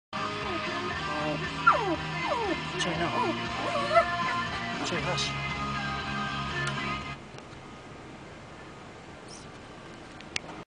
Fox in Downtown Denver
We were driving to a friends house in Denver and were surprised to see this little guy coming to visit also.